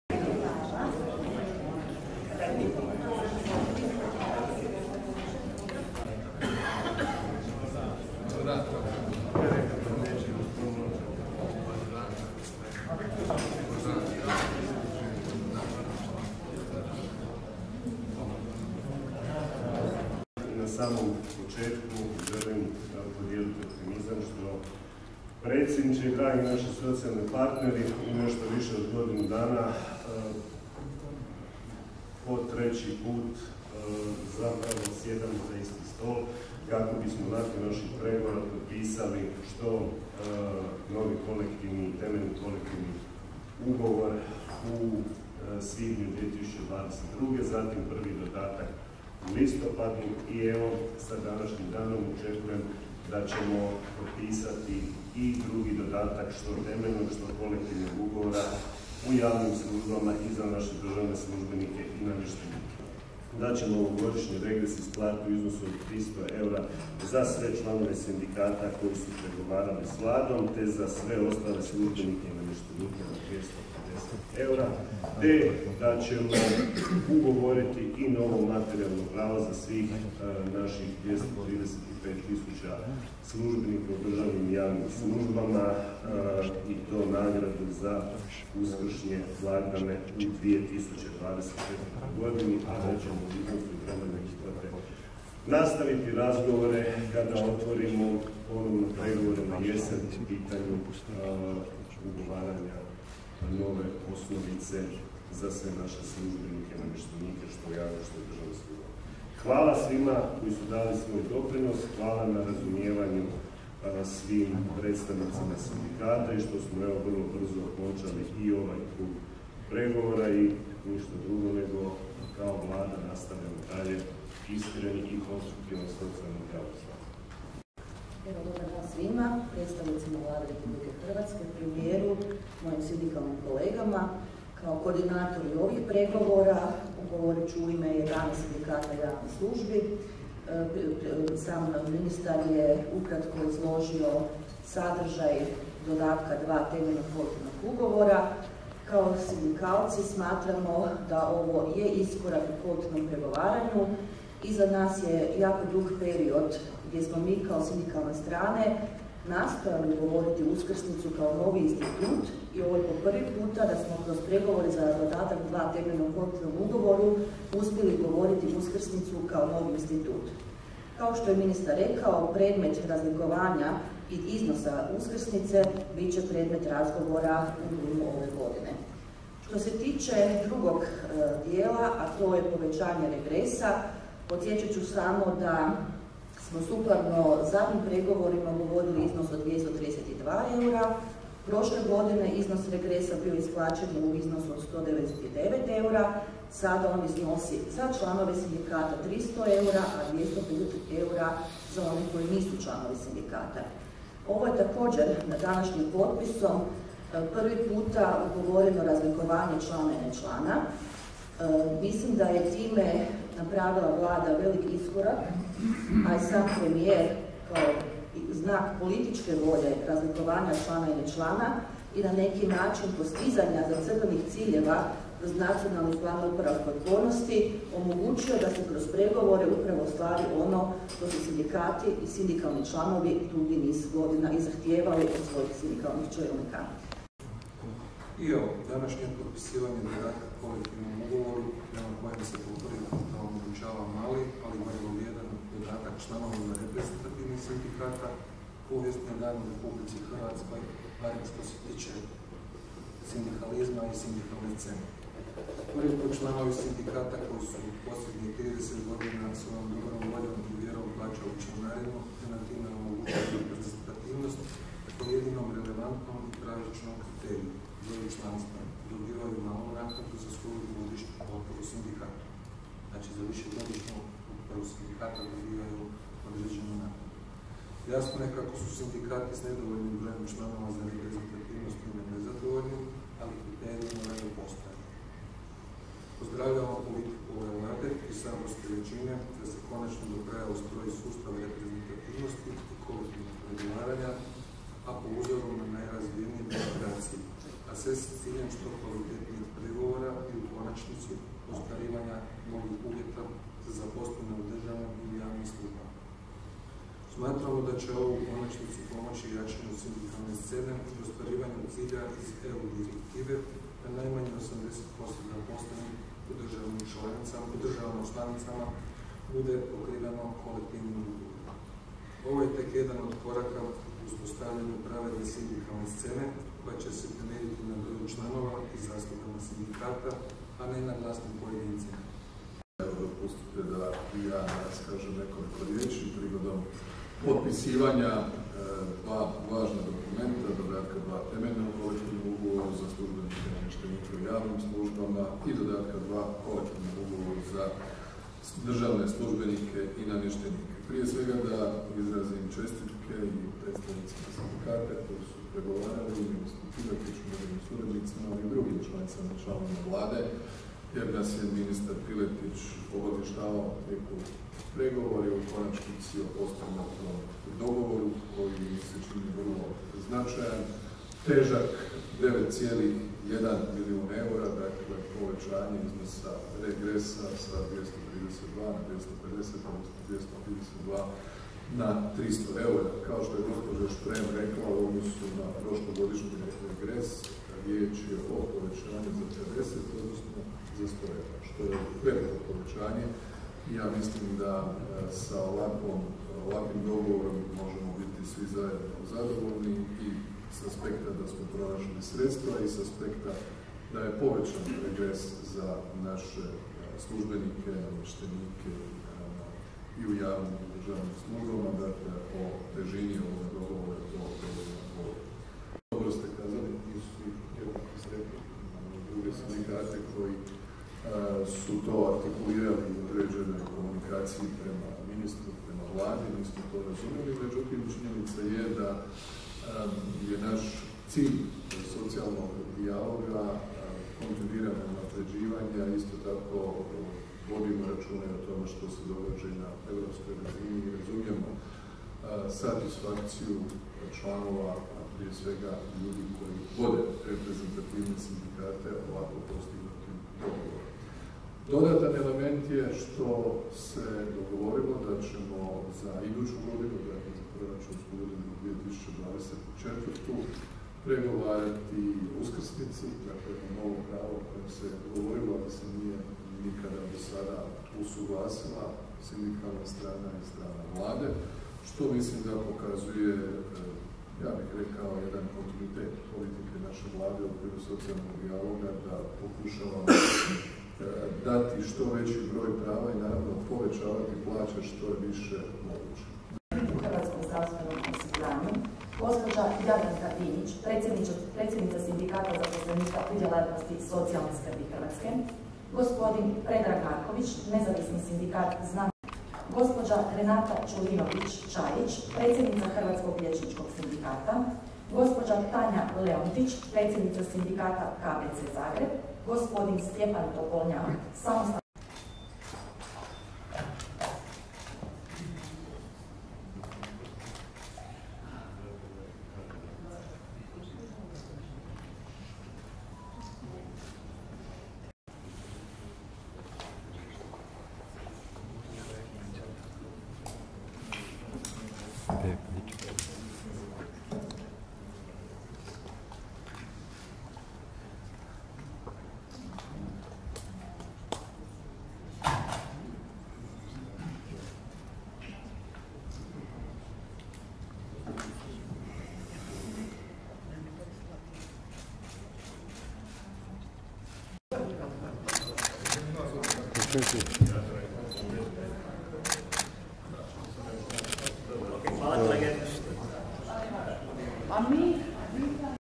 Sindikati i Vlada potpisali dodatak kolektivnom ugovoru
U Nacionalnoj i sveučilišnoj knjižnici svečano je potpisan Dodatak 2. Temeljnom kolektivnom ugovoru za službenike i namještenike u javnim i državnim službama.